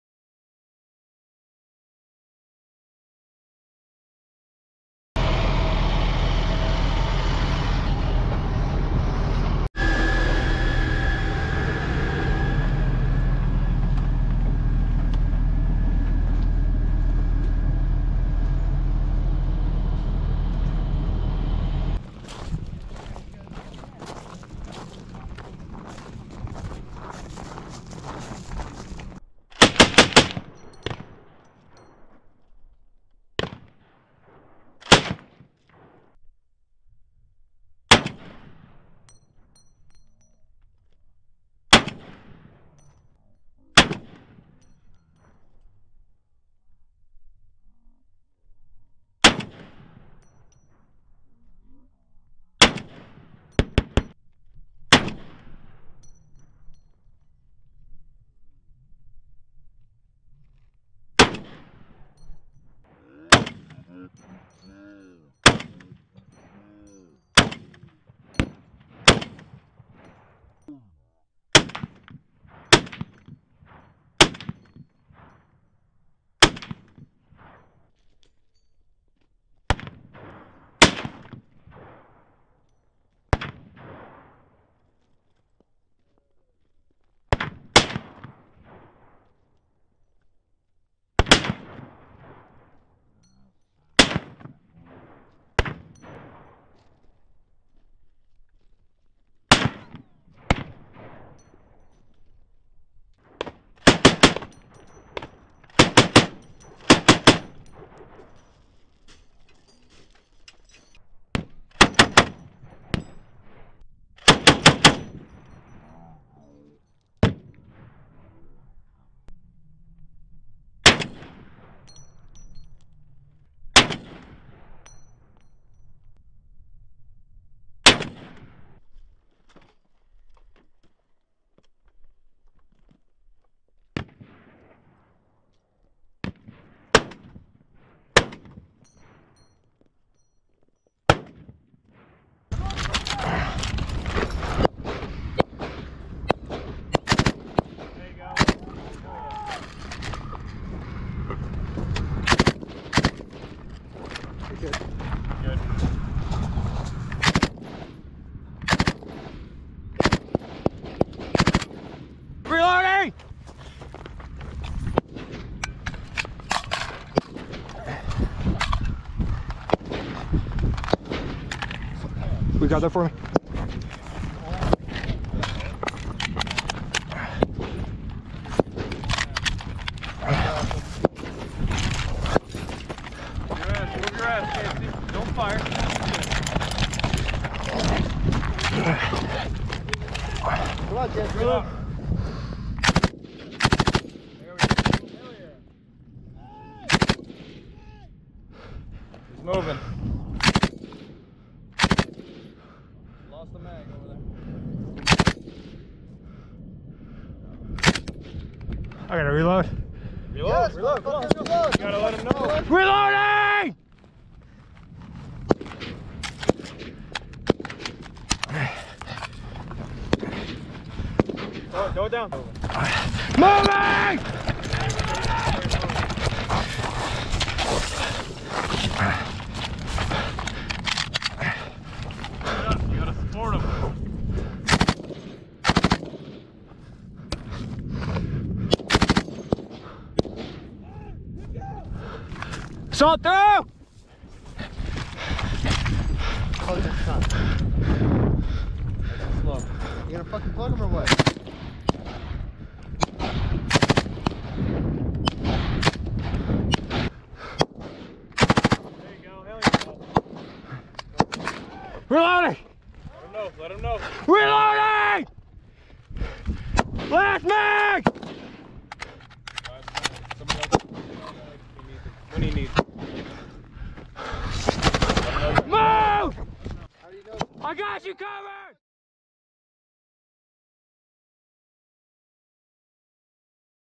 U.S. Soldiers Practice Live Fire Drills In Iraq (2020)
U.S. Army Soldiers conduct team-live fire drills at Al Asad Air Base in Iraq on February 5, 2020. Unit - 1st Battalion, 5th Infantry Regiment (Charger Company), 1st Brigade Combat Team, 25th Infantry Division.
Film Credits: U.S. Army Video by Spc. Derek Mustard, Combined Joint Task Force - Operation Inherent Resolve